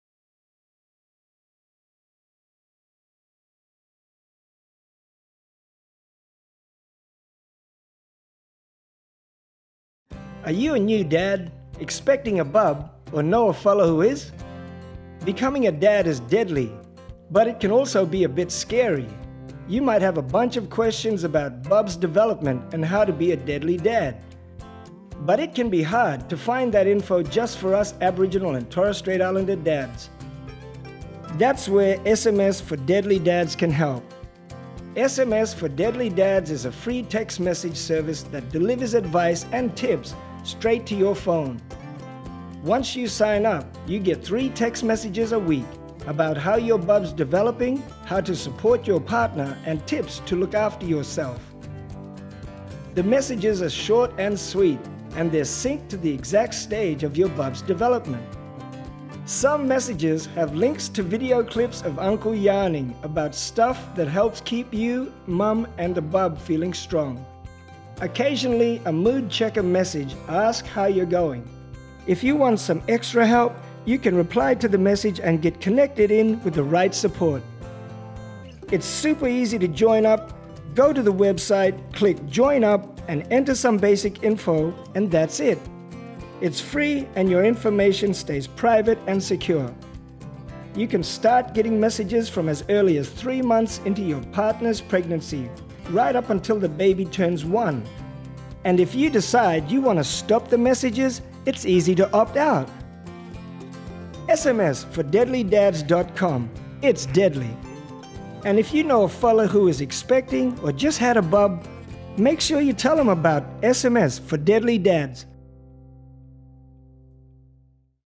SMS4DeadlyDads – What it's about & how to join up
SMS4DeadlyDads sends short texts with tips, info and support to soon-to-be and new First Nations dads. It’s a FREE service available to dads all around Australia.

SMS4DeadlyDads was first developed as a research project at the University of Newcastle (SMS4dads.com)

The messages have been co-designed in consultation with an Advisory Group of senior First Nations men representing Aboriginal Controlled Health organisations. First Nations dads also contributed to the message development.

Three text messages are sent to dads each week from 12 weeks into a pregnancy up until bub turns one. The messages are brief and to the point and talk about:

 •  Bonding and your baby's development
 •  Working as a team with your partner
 •  Looking after yourself and getting help if things get stressful

WHAT ARE THE MESSAGES LIKE?

Here's a couple of example messages:

4DAD: "You can start talking with me before I am born dad. Yarn with me about our mob and tell me stories about our culture!"

4DAD: "It isn't true that being a mum is natural and just gut instinct. Nearly all mums need support, especially if things aren’t going well."

4DAD: "I’m nearly as long as a footy now Dad. My legs are growing and I might try shake-a-leg. Imagine that!"

SMS4DeadlyDads.com
Join up to receive texts with tips, info and links to your phone just when you and your baby need it.